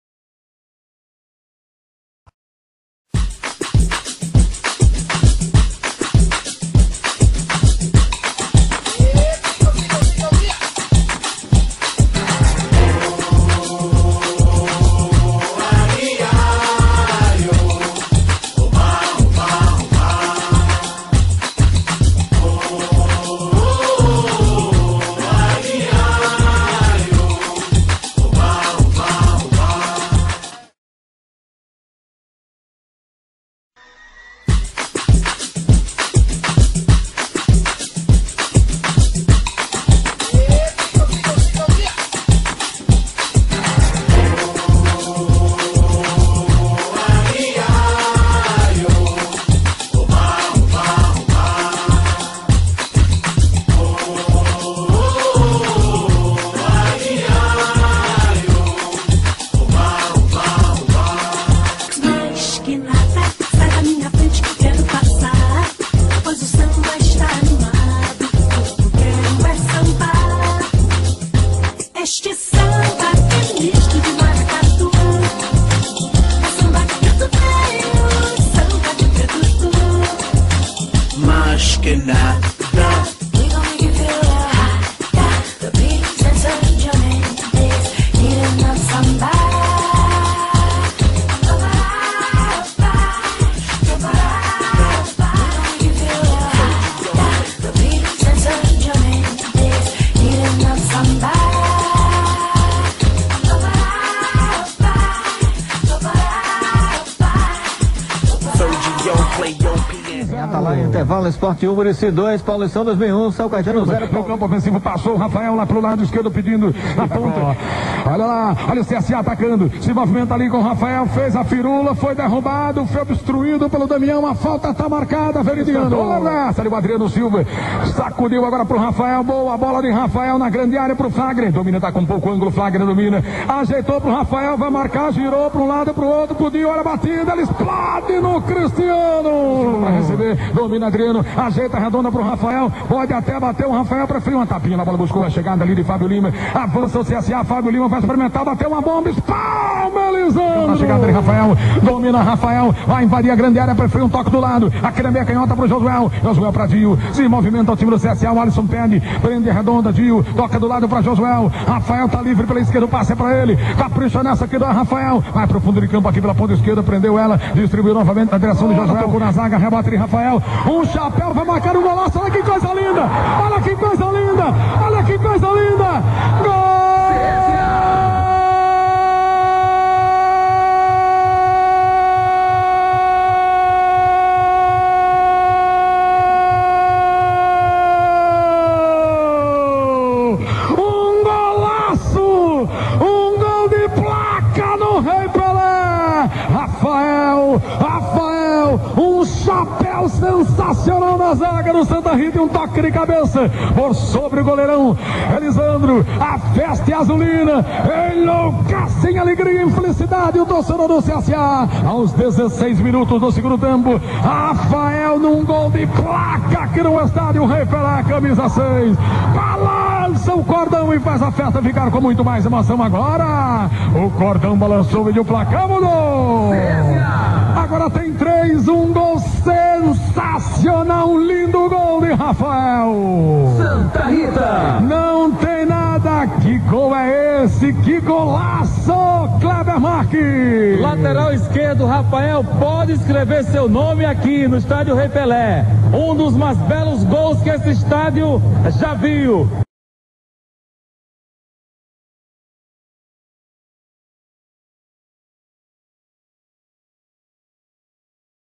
RAFINHA - CSA x Santa Rita - Alogoano 2011
Rafinha
Rafael Ricardo dos Santos Simões
Rio de Janeiro/Brasil
06/10/1983
1,91/5'7'' Altura
66kg/145lb Peso
Casado
Clubes:
S.C. Corinthians Alagoano 2009
Centro Sportivo Alagoano CSA 2009
Americano Futebol Clube 2010
Centro Sportivo Alagoano CSA 2011

Apoio - Pombal Sports